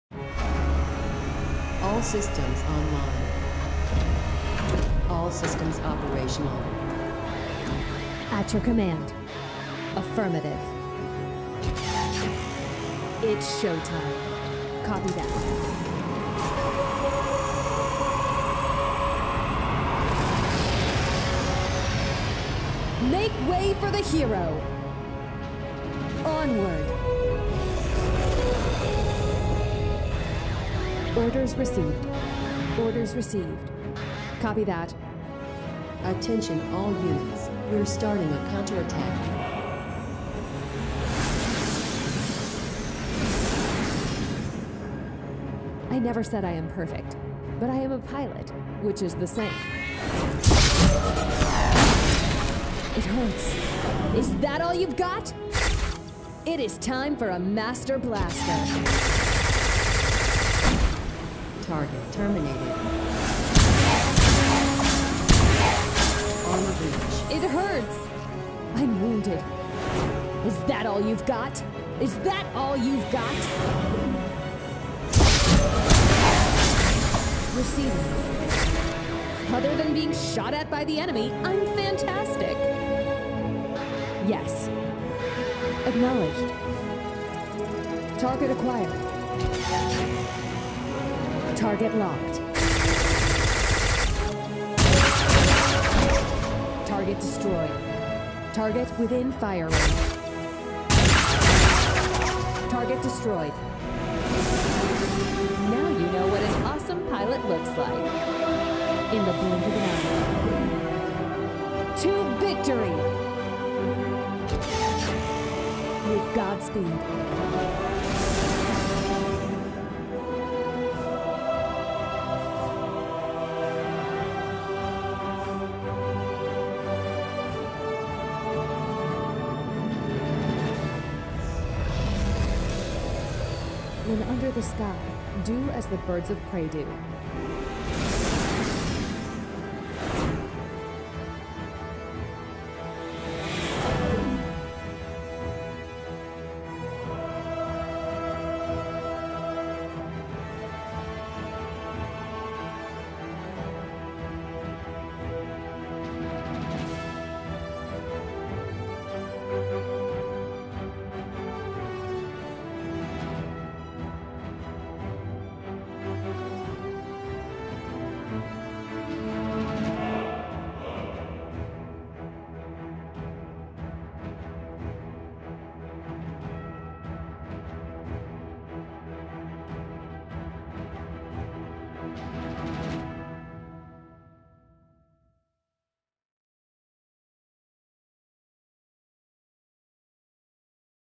Female Pilot / Soldier Voice Over Narrations vol.1
This collection contains the high-quality female voice performed by professional voice over performer.

This package contains 37 reusable phrases (some in many variations - 154 phrases total) spoken by excellent narrator.

Created for the most demanding production and reusable in future projects.

For production of this package, we used the highest quality gentle and expensive reverb & analog devices from reputable audio companies, to provide the best voice experience known from top movies and AAA + games.

This package will give life your female pilot or soldier.

Narrations in demos are without any additional effects or post processing.